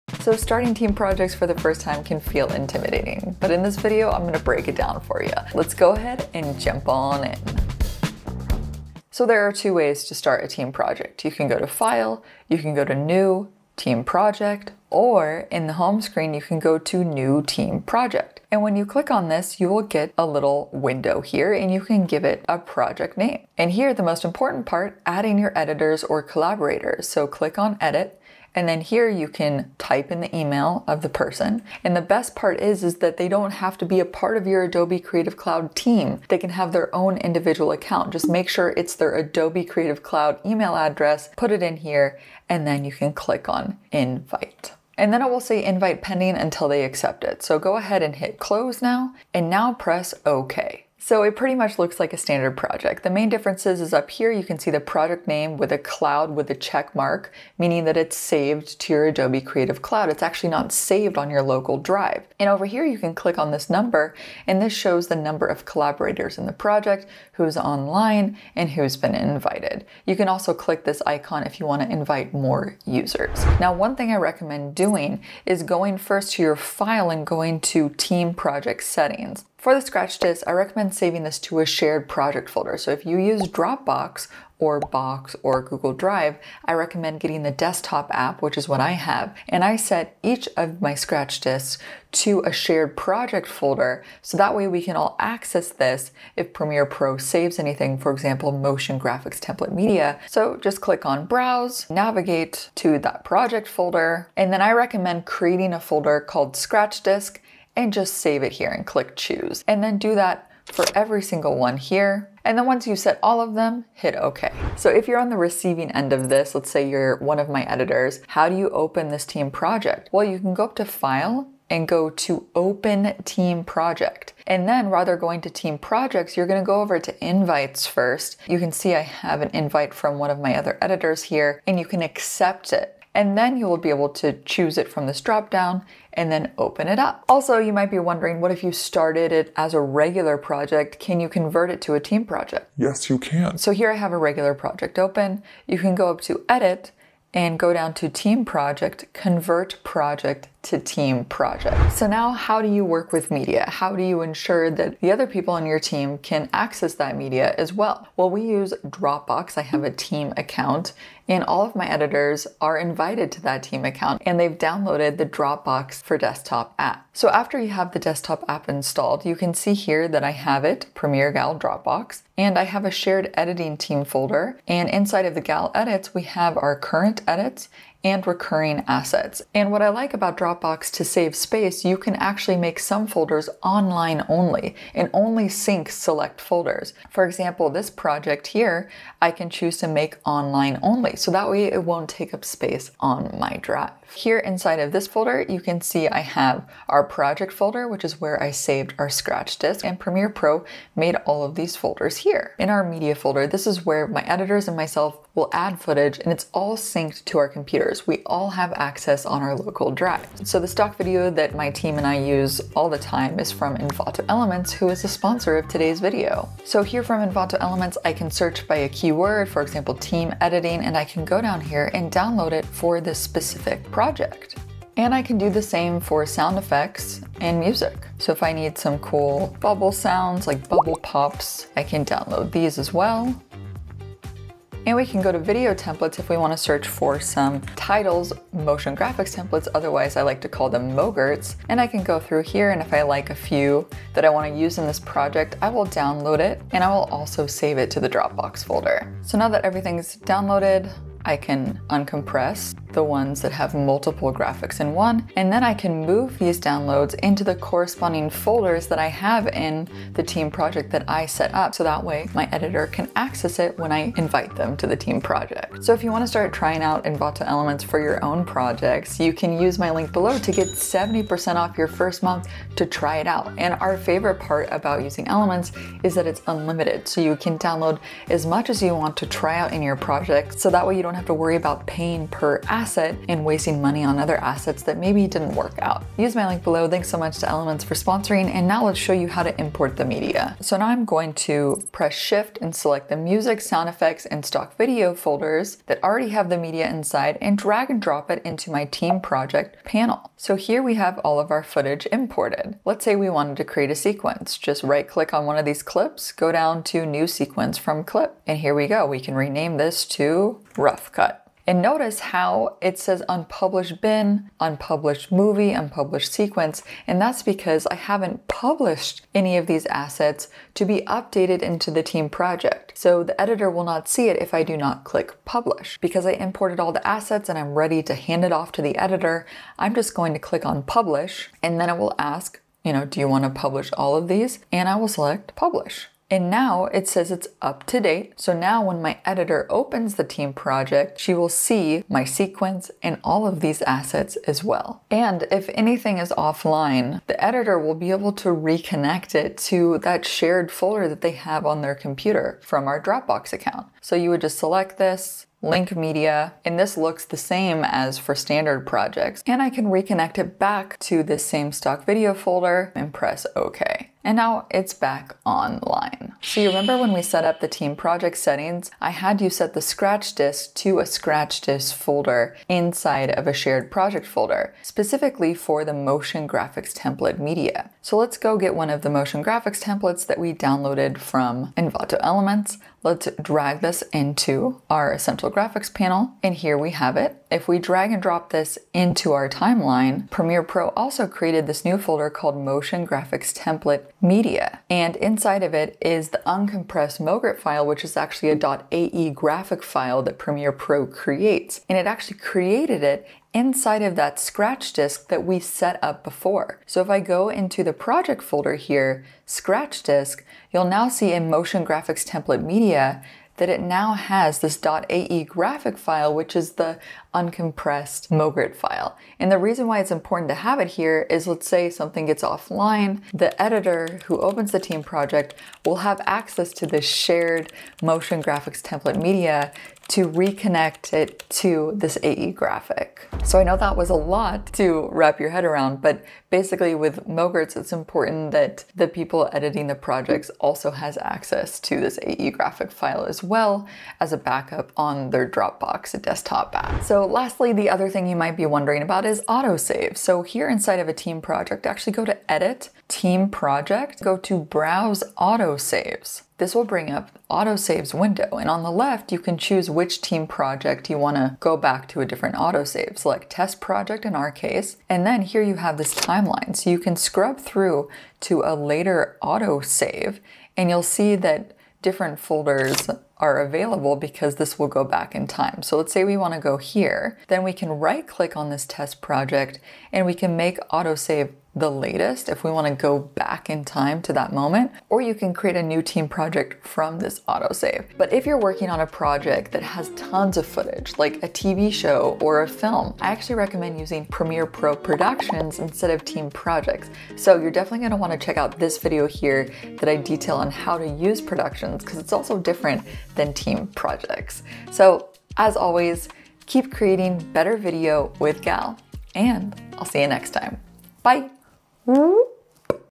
What are Premiere Pro Team Projects? (2023 Update and Guide)
Want to take your video editing to the next level?

ABOUT TUTORIAL
Team Projects are now available to use with individual Creative Cloud Accounts, this means you can share your project files and assets over the cloud and collaborate with multiple editors or 1 additional editor (from anywhere), with a good internet connection of course! My team and I use Dropbox to share file assets with each other and Team Projects to edit and share projects that we can open simultaneously.

PREMIERE GAL TOOLS
TIMECODE CHAPTERS
0:00 Intro
0:10 How to Create a Team Project
1:18 Media Management
1:58 How to Open a Team Project
2:29 How to Convert a Regular Project into a Team Project
2:45 Using Cloud Storage like Dropbox to Share Media
3:30 Uploading and Sharing Files from Envato Elements
4:55 Editing in Team Projects
5:43 Reconnecting Offline Media
6:00 Importance of the Scratch Disk with Motion Graphics Templates
7:29 Auto Save in Team Projects

Edited by Stephanie Yu
Thumbnail by Rikard Tholen & Hankok Design